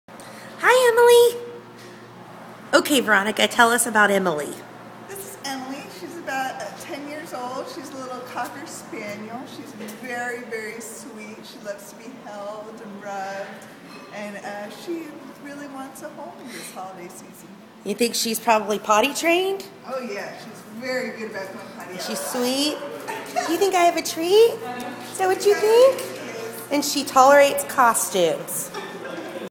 Meet Emily a Spaniel American Cocker currently available for adoption at Petango.com! 12/9/2013 3:3
Meet sweet Emily. She is an adorable senior cocker spaniel who was rescued at the last second from another shelter. We knew it might be harder to find someone willing to adopt an older dog but one look into Emily's lovable eyes and we just knew we had to try. Emily is small for a cocker, she doesn't mind being held and kind of enjoys getting brushed and pampered. This little girl deserves a loving family for whatever time she has left. Don't overlook this beauty because of her age, she still has lots of love to give. Come meet Emily today!